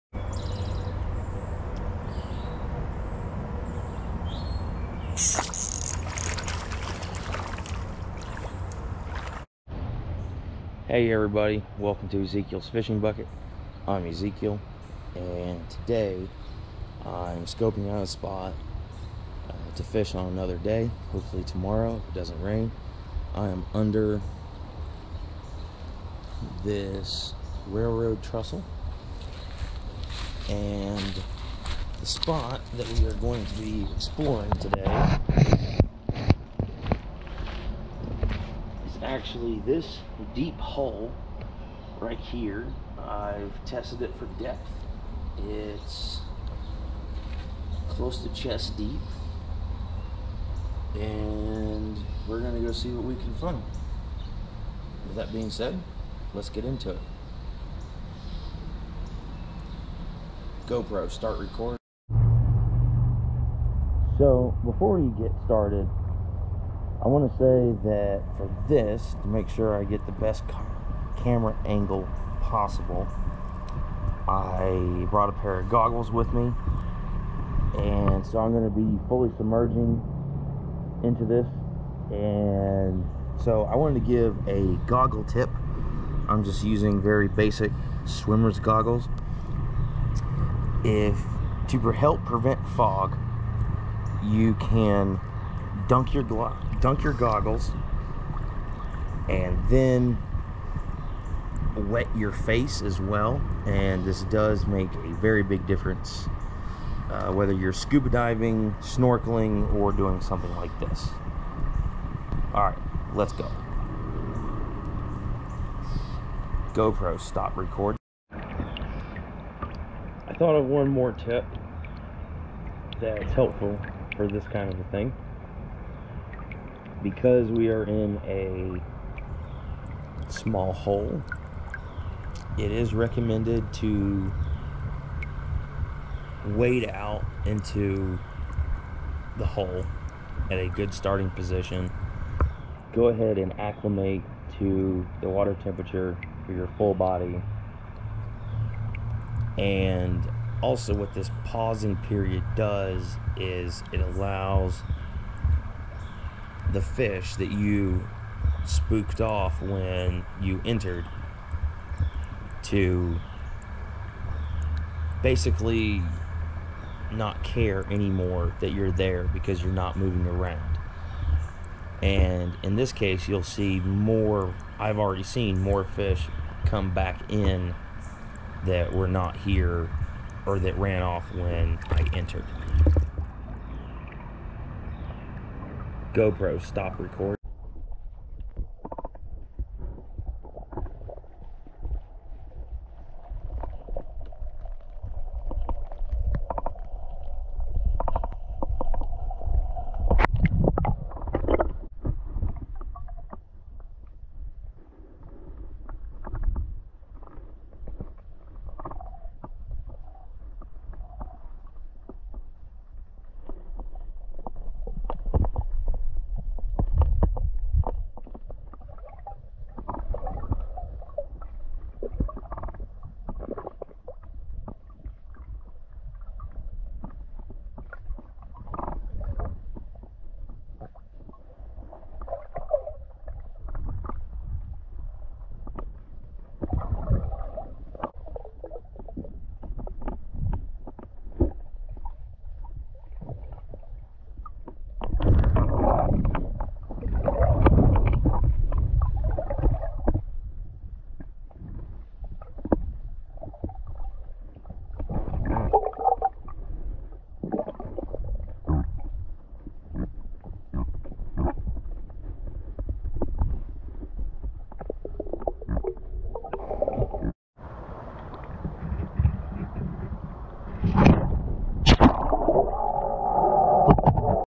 Spying into a fishing whole to see what I can find.
Brook trout underwater view. Gopro hero 9.